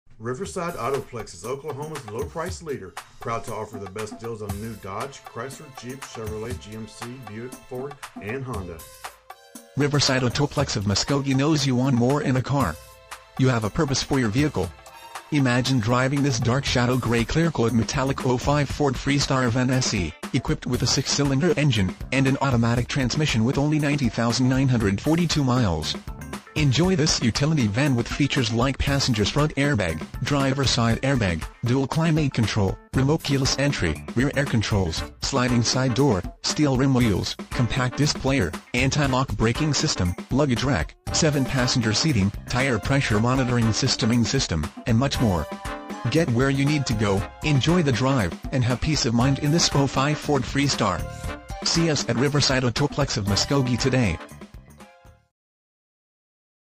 2005 Ford Freestar Riverside Autoplex of Muskogee Muskogee, OK 74401
Ford Freestar Riverside Autoplex of Muskogee knows you want more in a car. You have a purpose for your vehicle. Imagine driving this Dark Shadow Grey Clearcoat Metallic Ford Freestar Van, equipped with a 6 Cylinder engine and an automatic transmission with 90,942 miles. Enjoy this utility van with features like Passenger's Front Airbag, Driver Side Air Bag, Dual Climate Control, Privacy Glass, Remote Keyless Entry, Rear Air Controls, Sliding Side Door, Steel Rim Wheels, Compact Disc Player, Anti-Lock Braking System, Luggage Rack, Tinted Glass, 7 Passenger Seating, Tire Pressure Monitoring System, Cruise Control, 3rd Row Seats, Rear Bench Seat, Anti-Theft Alarm, and much more. Get where you need to go, enjoy the drive, and have peace of mind in this Ford Freestar. See us at Riverside Autoplex of Muskogee today!
MILES: 90,942
VIN:2FMZA51635BA05313

1711 West Shawnee
Muskogee, Oklahoma 74401